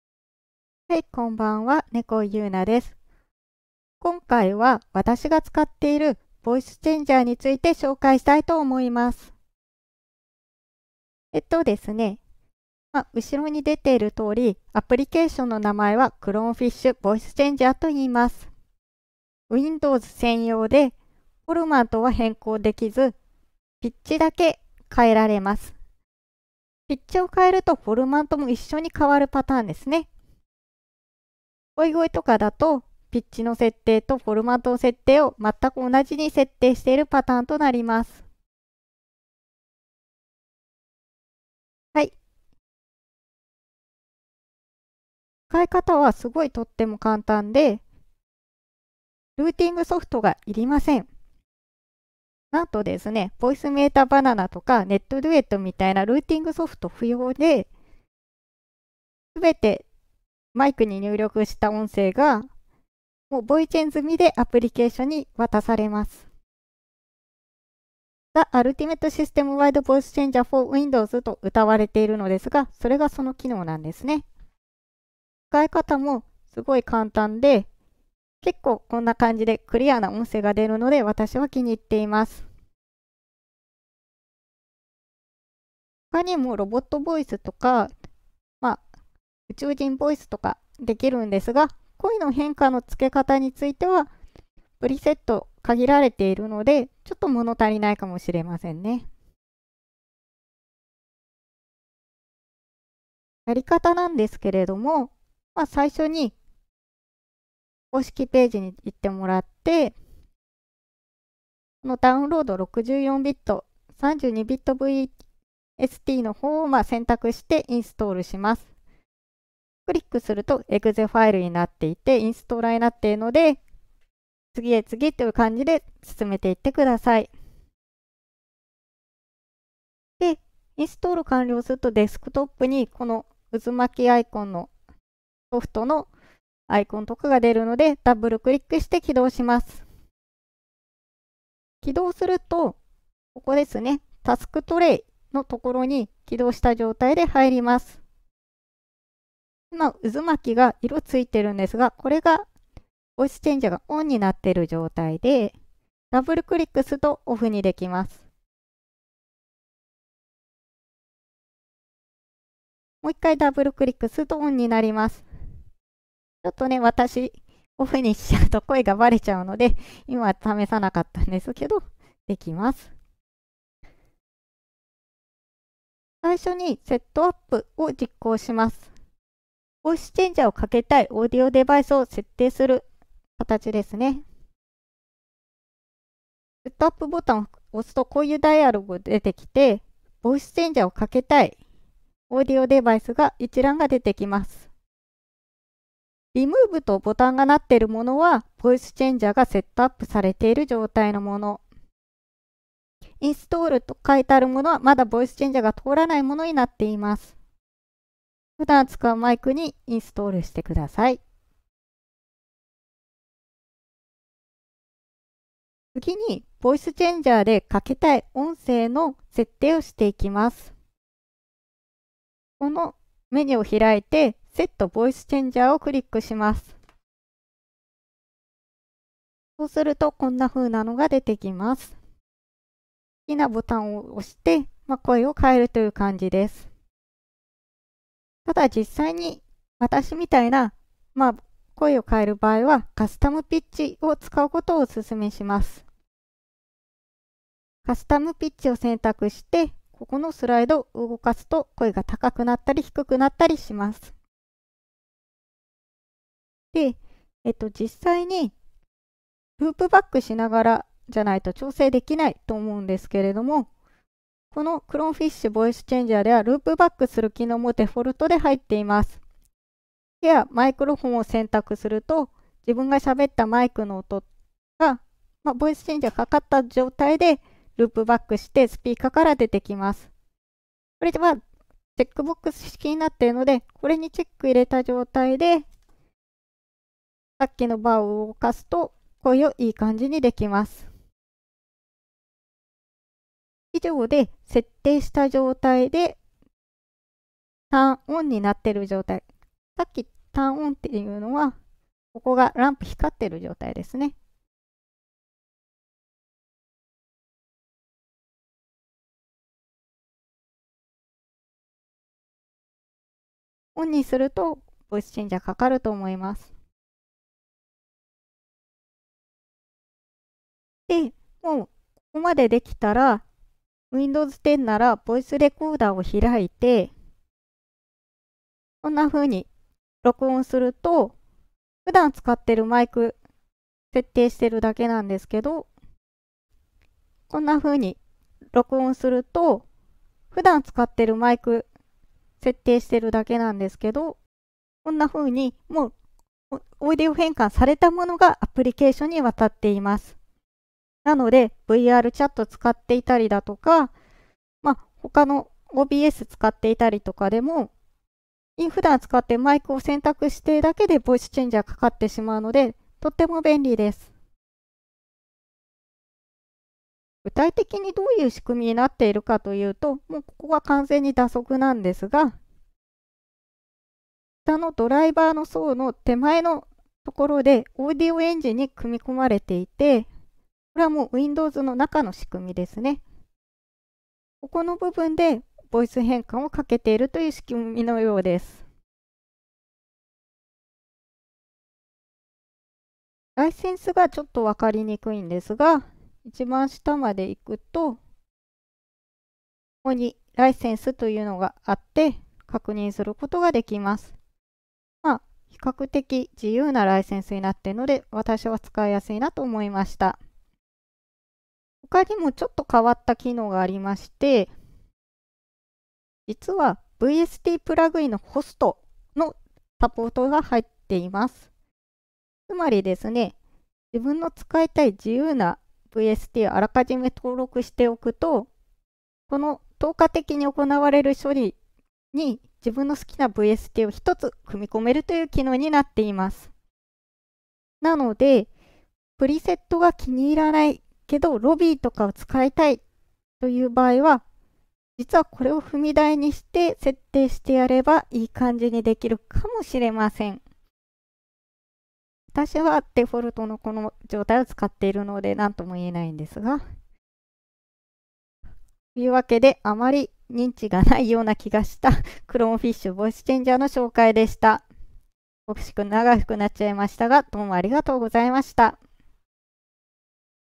ClownfishVoiceChangerの紹介
私が普段使っているボイスチェンジャー ClownfishVoiceChangerの紹介です。ピッチの変更しかできませんが低遅延・高音質です。また、入力した音声が変換されたものがアプリケーションに渡されます。そのため、VoiceMeeterBananaのような仮想ミキサー・ルーティングソフトが不要なため使い勝手がよくVRChat,バーチャルキャスト,youtuber活動とも相性が良いです。